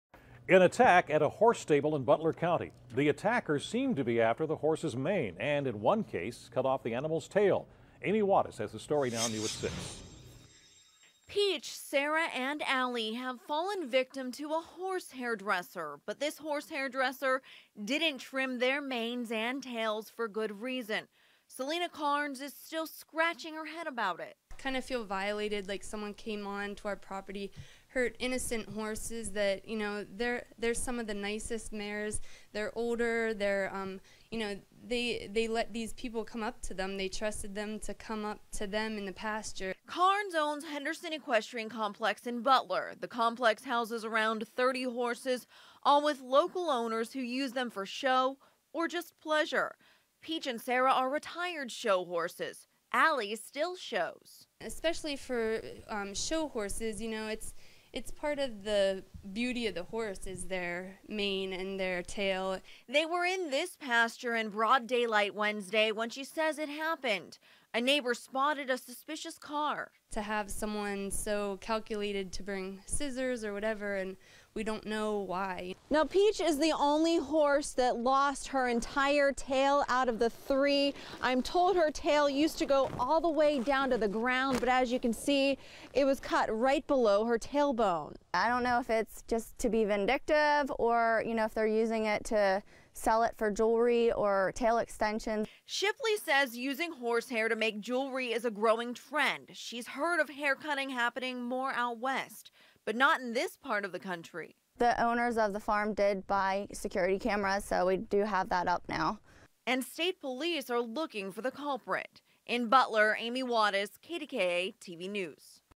Hair Thief Chops Manes, Tails From Butler Horses
Police are investigating an attack at a horse stable in Butler County; KDKA's Amy Wadas reports. FULL STORY